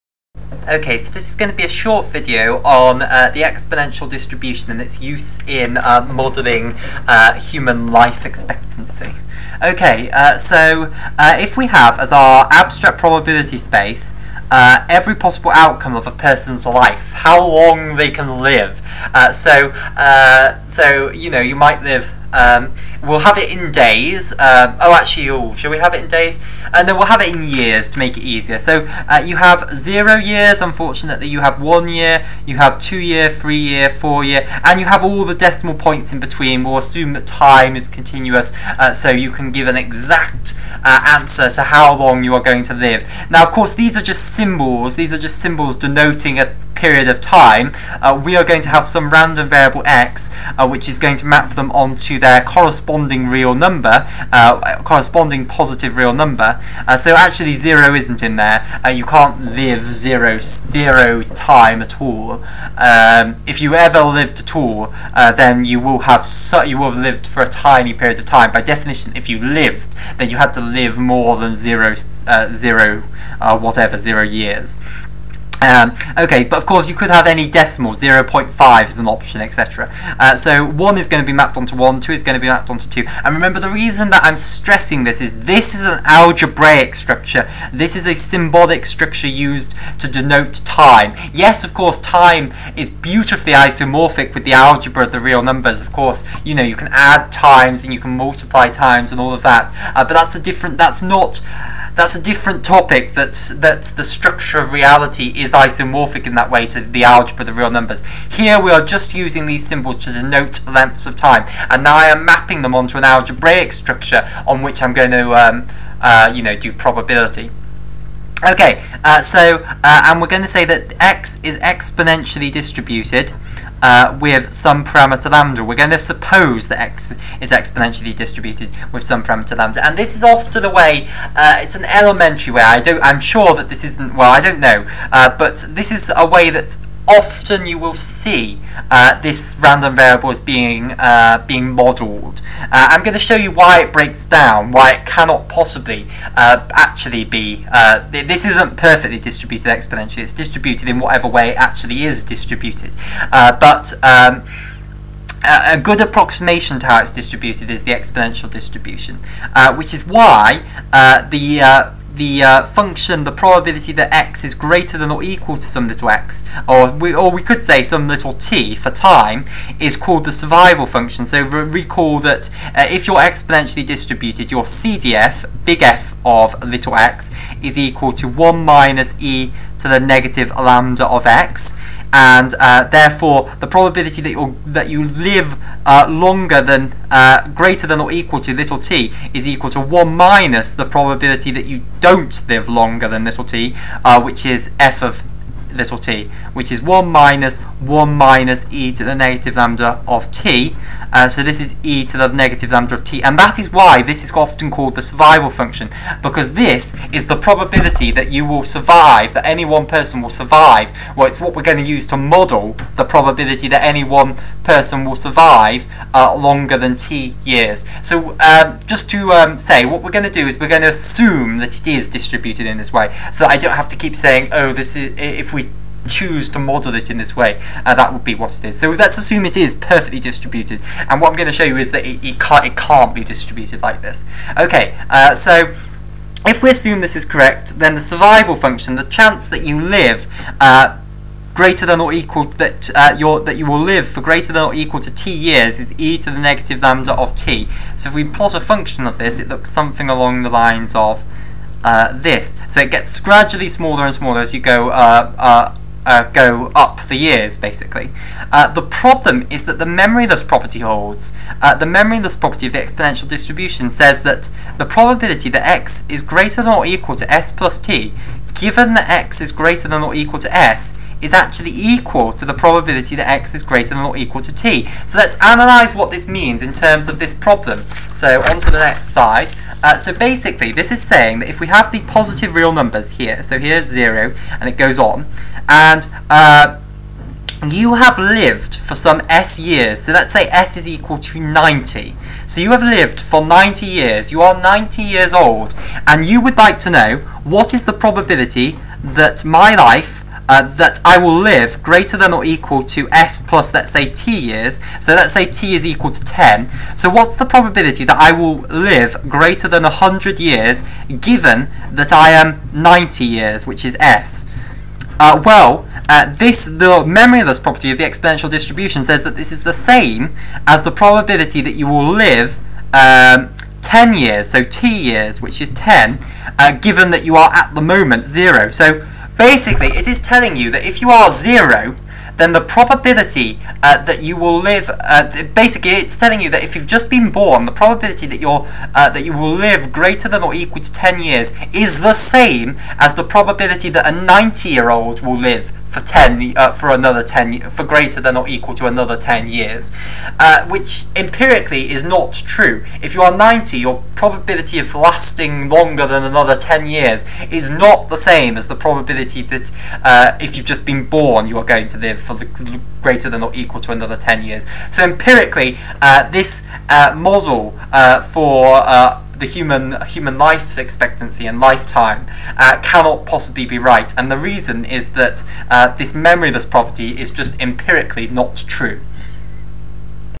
A Demographic Application of the Exponential Distribution
We examine the downfall of the exponential distribution in modelling human lifetime.